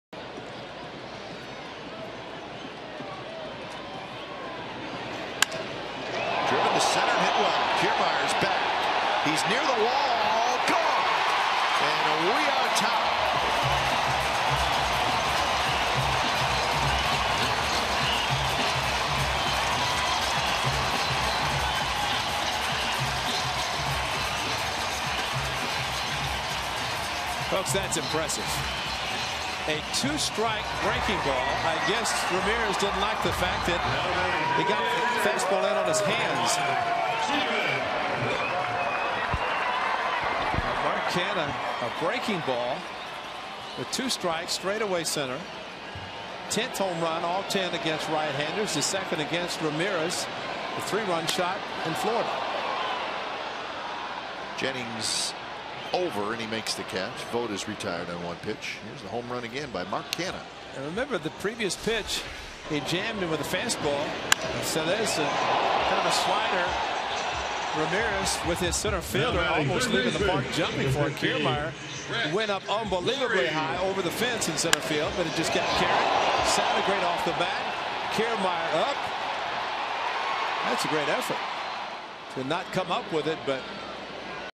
TB@OAK: Canha ties the game with solo shot to center
8/22/15: Mark Canha belts a solo home run to straightaway center field to tie the game at 2 in the 6th inning